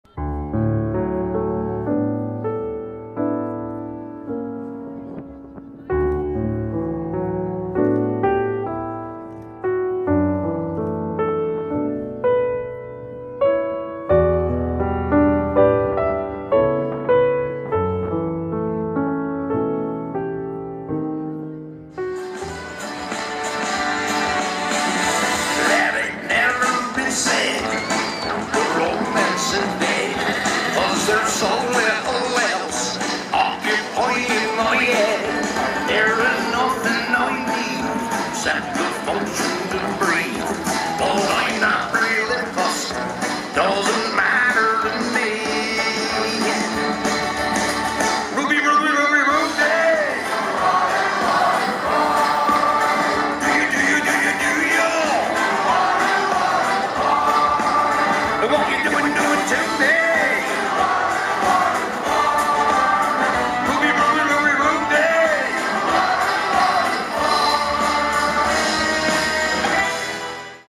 Bristol Balloons
Hot Air Balloons over Clifton suspension bridge and in  Ashton Court!  Bristol Balloon Fiesta!